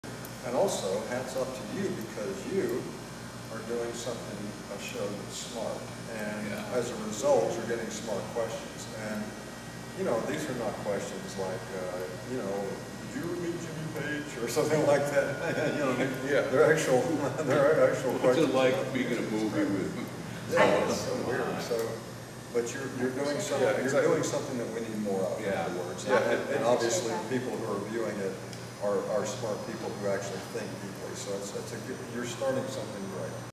Billy Bob Thornton & The Boxmasters Endorsement Why you should tune in to Tennessee Sound Review.
The Boxmasters and Billy Bob Thornton interview clip, giving their feedback on Tennessee Sound Review, TSRtv / Tour Talks! Many thanks to Billy, J.D and Teddy for your time and kind words.